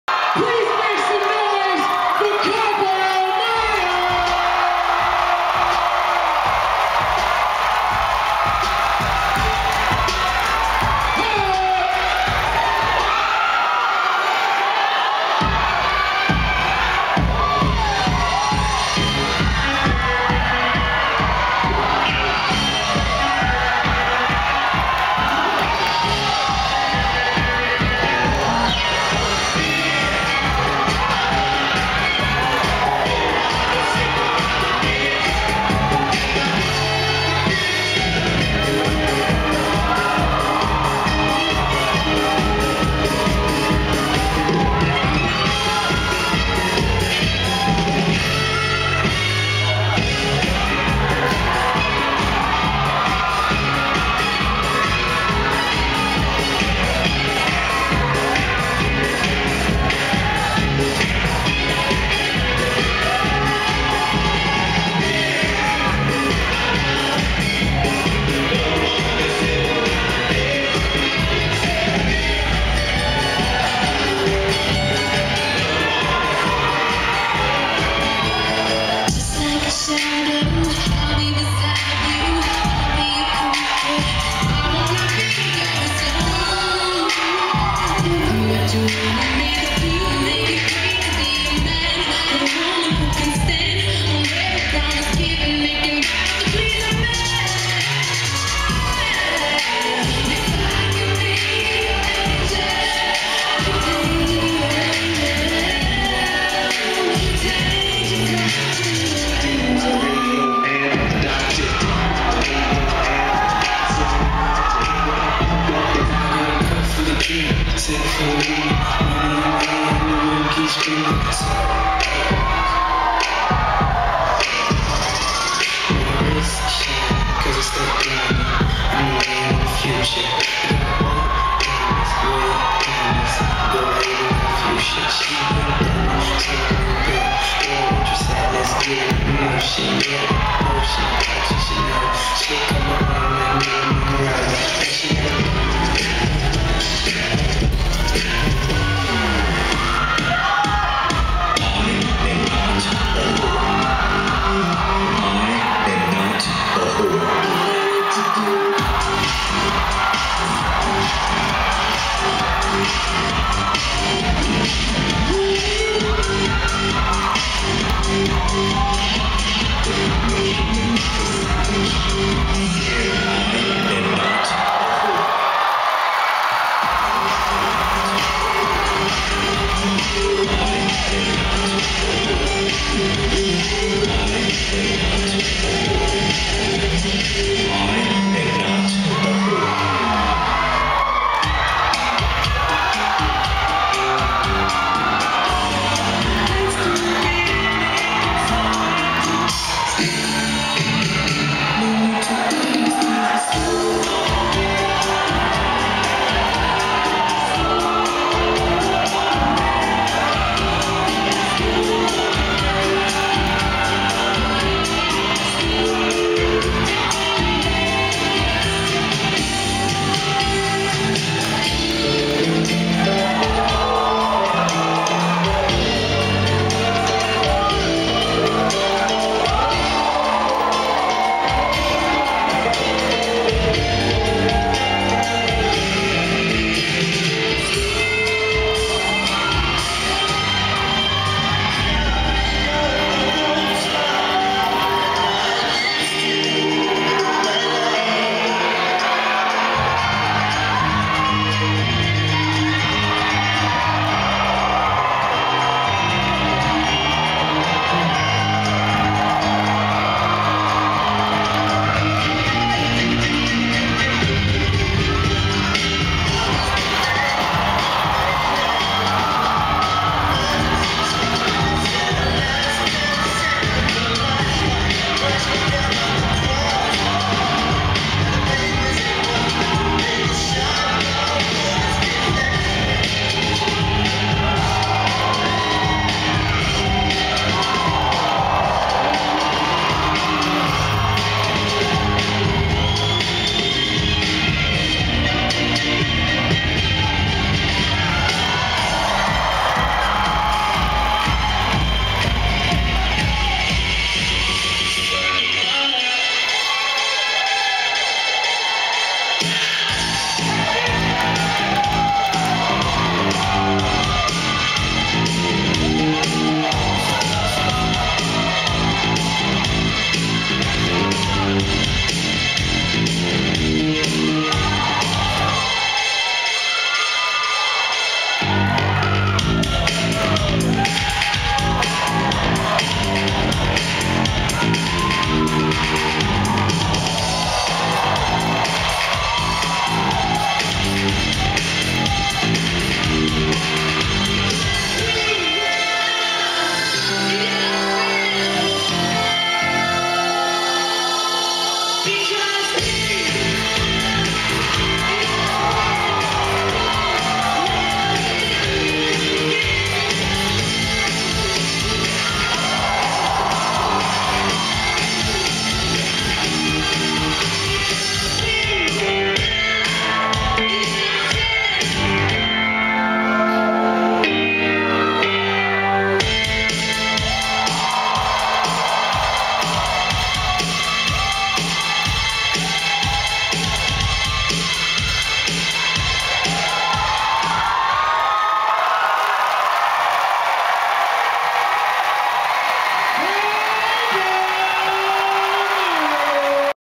KABA Modern - VIBE XIV
Kaba Modern at VIBE 14
January 25, 2009
UC Irvine's Bren Events Center

Coordinators:
Allison Vicencio
Andrew Simbol
Geo Lee

Choreographers (in order):
Hikari Murakami
Yosuke Murakami
Darina Littleton
Jonah Aki
Shawn Duong
Geo Lee / Darren Wong
Shawn Duong / DJ Casis / Andrew Simbol / Jenny Valles / Jon Aldanese

*Music mix edited by Andrew Simbol